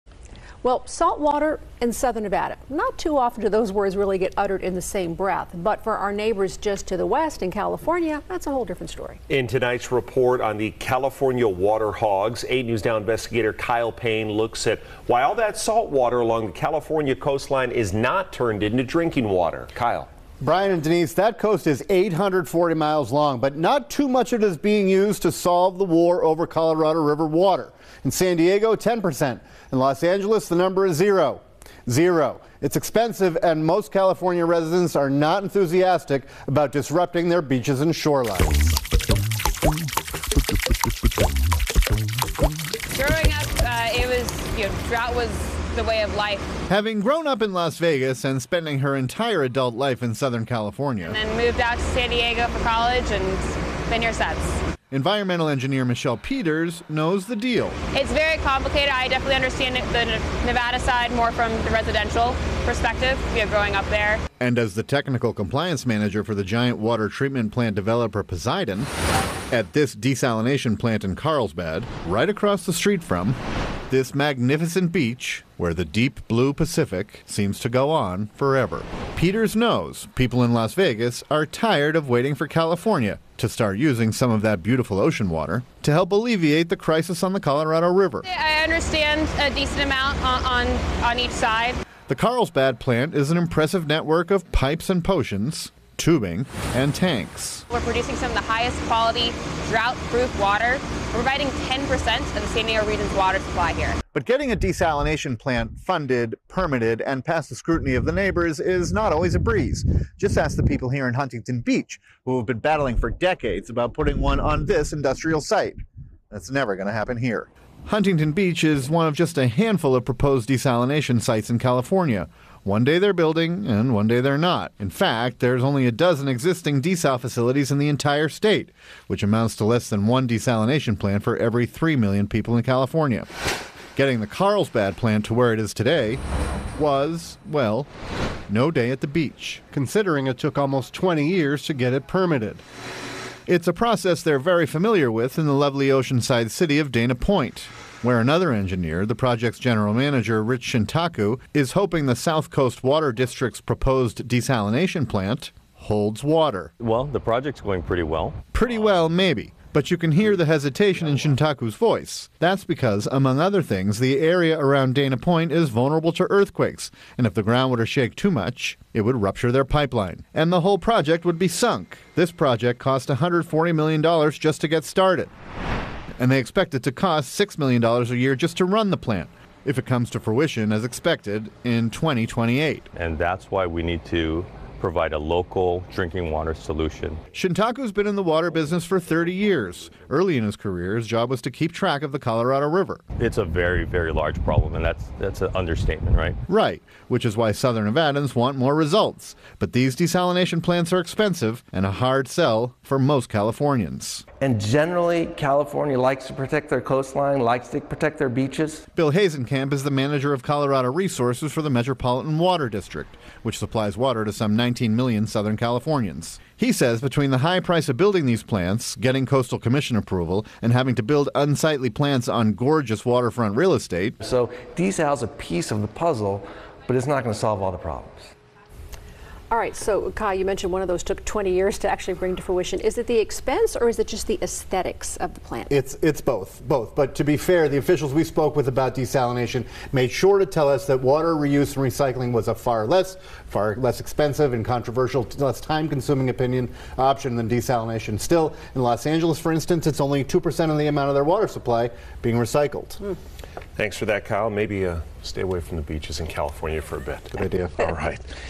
What is desalination, how can it end war over Colorado River?
The California coastline, a seemingly limitless supply of ocean is at the forefront of a lot of resistance statewide to turn the Pacific salt water into drinking water.